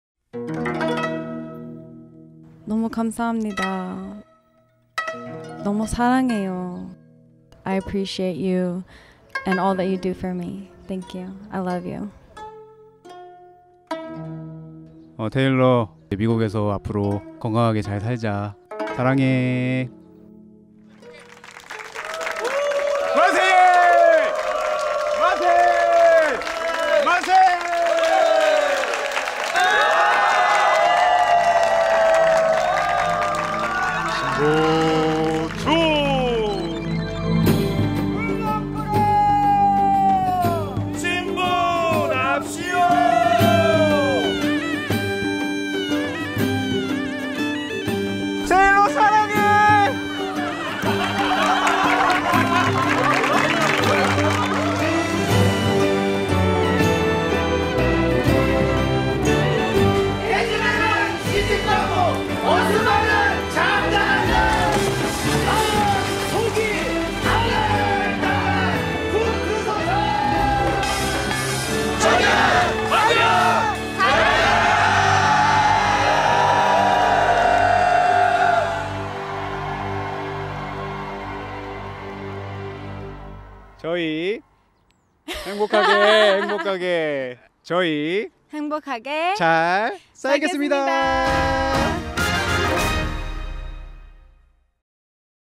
한국식 전통 혼례 제대로 맛본 미국 와이프 [예고편]
안녕하세요 검보와 김치입니다! 그동안 잘 지내셨을까요?

오랜만에 영상으로 찾아뵙게 되었습니다 ㅎㅎ

제이와 테일러는 한국에서 전통혼례를 마치고 2주간 휴가를 보낸 후 미국에 귀국하였어요.

정신없이 하루하루를 보내는 와중에 한국 전통혼례 영상을 제작해 주셨던
＇주노무비＇에서 예고편을 보내주셨습니다!!!

이렇게 퀄리티 높은 예고편을 보내주셔서 우리 구독자분들께 안보여 드릴 수가 없겠더라고요 ㅎㅎㅎ

결혼식 당일에도 아주 아름답고 멋지게 연출해주시기 위해 노력해주신 게 기억에 많이 남습니다. 디테일한 묘사 그리고 특히 아름다운 각도와 선을 강조해서 찍어주시려고 신경을 엄청 써주시는게 느껴졌답니다ㅎㅎ 사진도 너무너무 예쁘게 잘 찍어주셨고요. 혹시나 다른 구독자 분들이 전통혼례를 하시게 된다면 저는 다시한번 주노무비를 선택 할 것 같습니다!! 다시한번 너무 감사합니다^^

저희 본식 영상은 편집이 완성되는데로 올리도록 하겠습니다 ㅎㅎ
조금만 기다려 주세요~

Hello everyone~ We've missed yall.

Here is a little teaser of what is to come from out time in Korea for our 2nd ceremony.

We had the best experience with Juno Movie during our wedding, who took all the photos as well as this video. They were so professional and easy to work with. I have to say that I think they captured the moments in the best ways possible. Especially those with our family and close friends.

We will have plenty more Korea-based content for you guys so be tuned for some future videos!

Just be a little patient with our editing since Jay is back at work again :) We promise to do our best to get more videos out soon.

Thank you guys for all of your love and support. :)